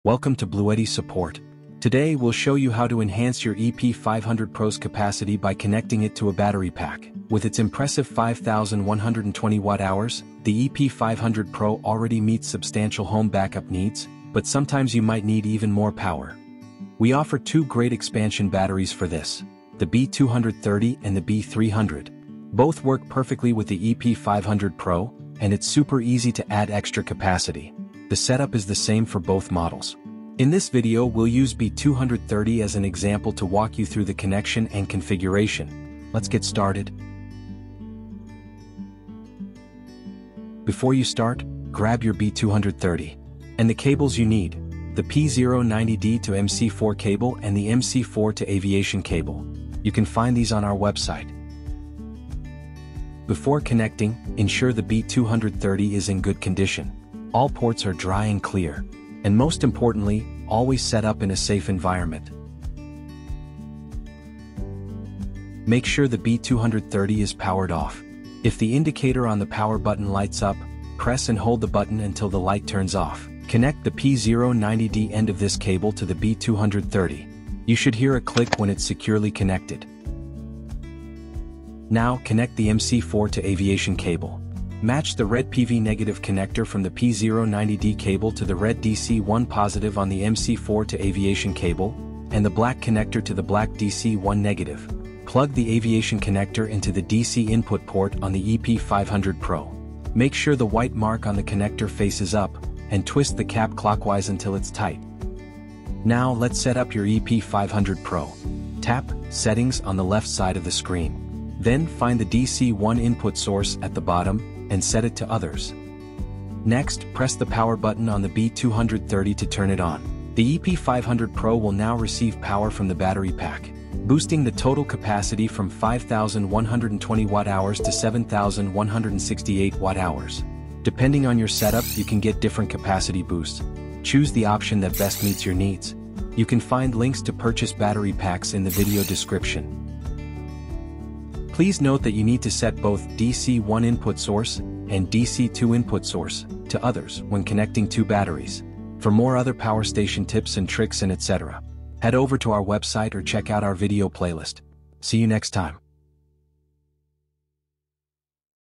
How to Expand the EP500Pro's Capacity
Description: Need more power for your EP500Pro? In this video, we'll show you how to expand the capacity of your BLUETTI EP500Pro using the B230 or B300 battery packs. Whether you're powering your home during an outage or extending your off-grid adventure, boosting the EP500Pro is quick and easy.